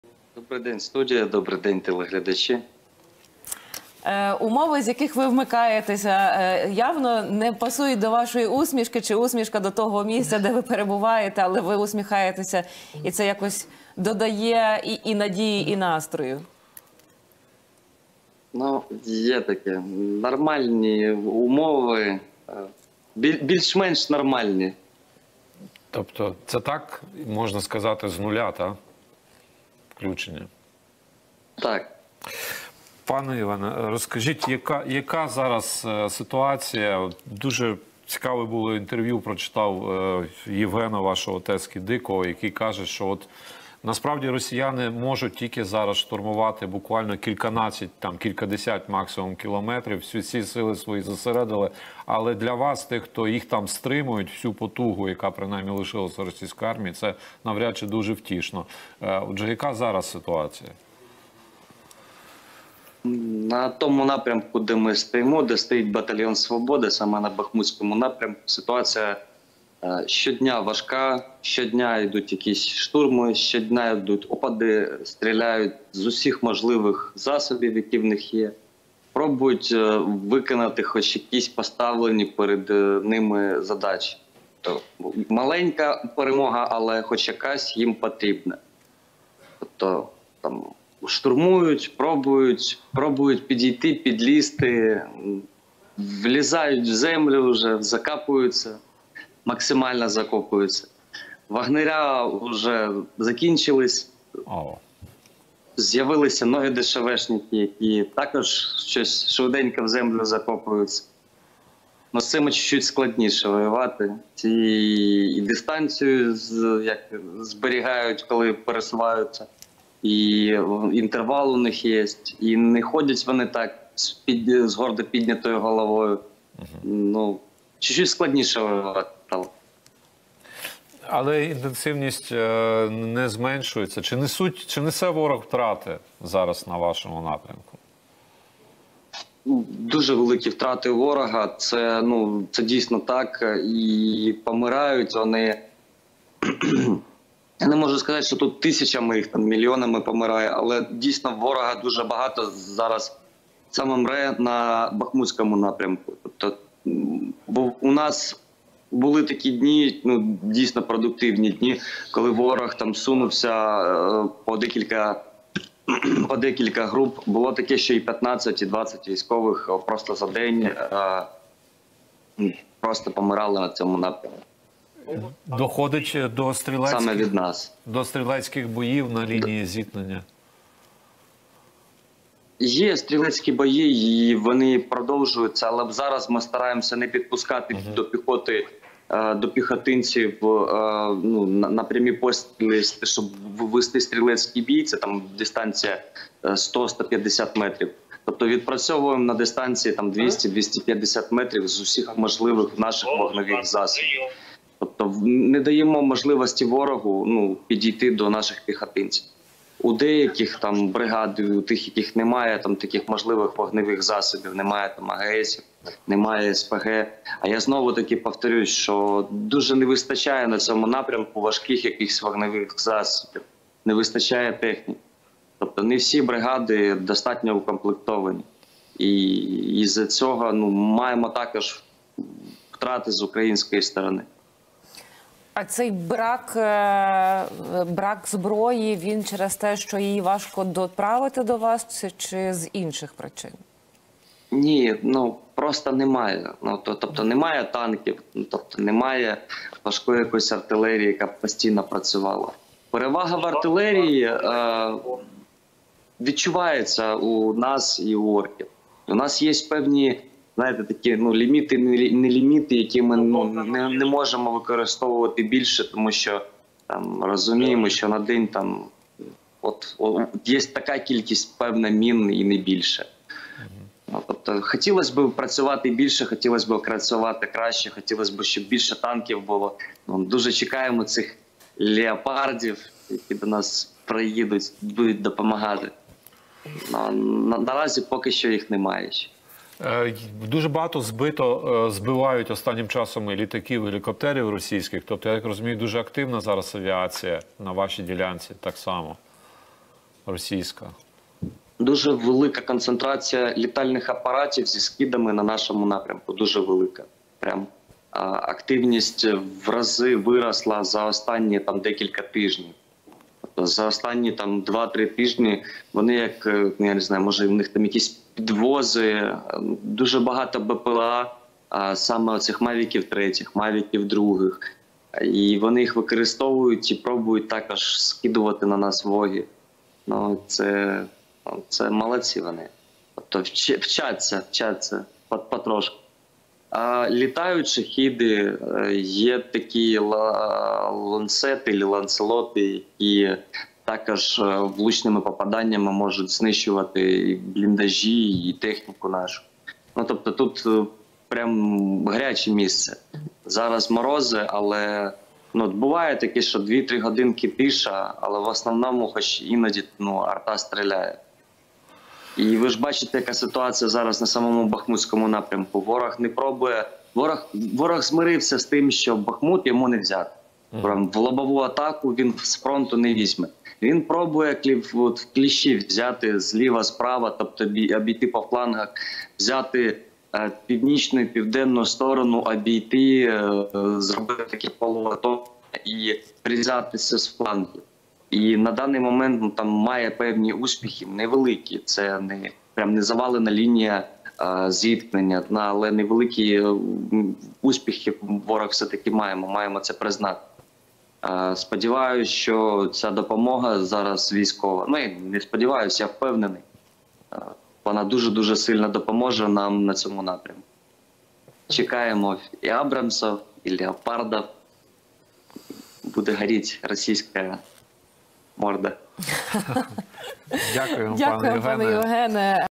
Битва під Бахмутом: фронтова ситуація вдень 28 січня — Євген Оропай / Легіон Свободи
З Бахмутського напрямку Євген Оропай, лейтенант «Легіону Свободи», командир роти 3-го батальйону оперативного призначення «Свобода» у складі 4-ї бригади оперативного призначення Національної гвардії України в етері телеканалу "Еспресо" 28 січня 2023 року.